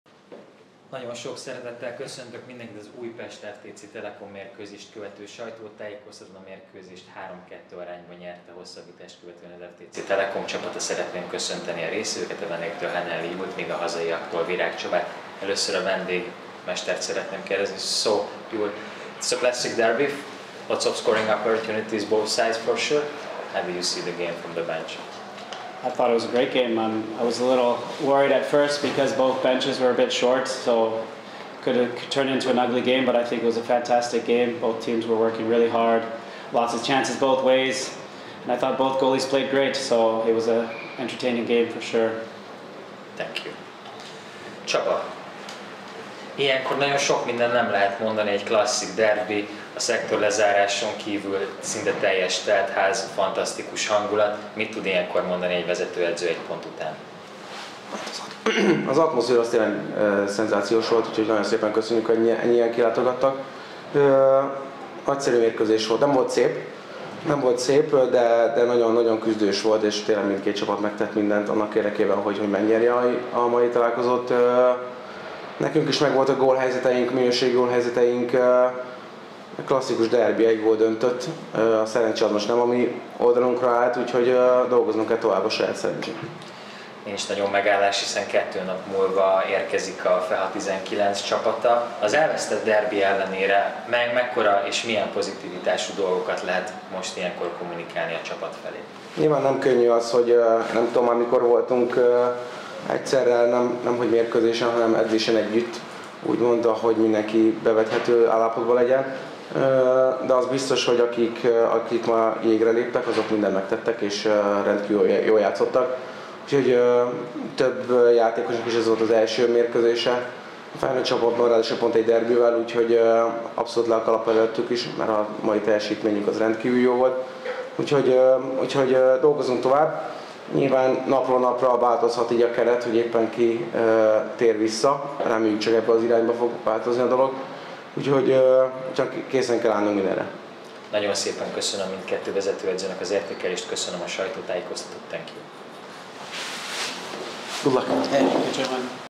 Sajtótájékoztató / 2022.12.28.
Az FTC-Telekom elleni mérkőzést követő sajtótájékoztató